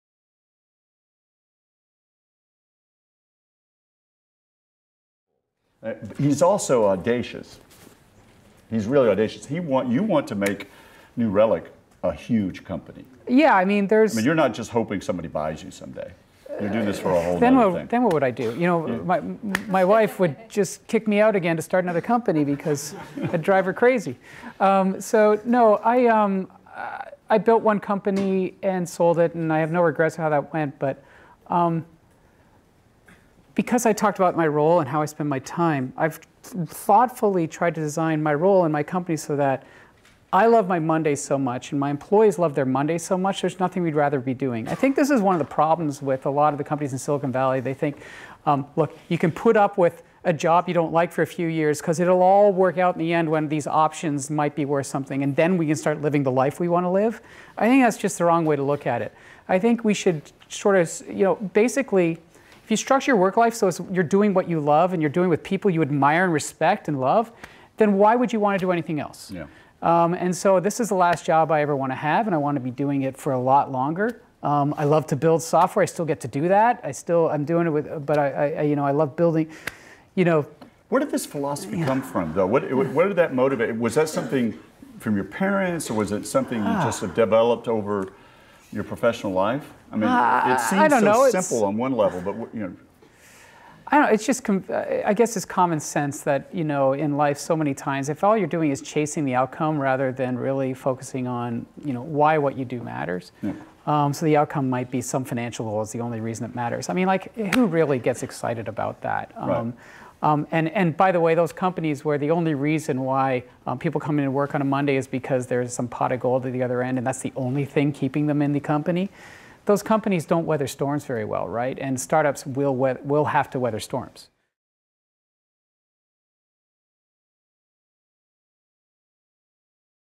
Lewis Cirne: Make the Work Your Reward
In conversation with Stanford Engineering Professor Tom Byers, Lew Cirne talks about founding New Relic out of pure passion for building software, not for the sake of financial gain. Cirne also underscores how work should be truly meaningful and not a means to a profitable end.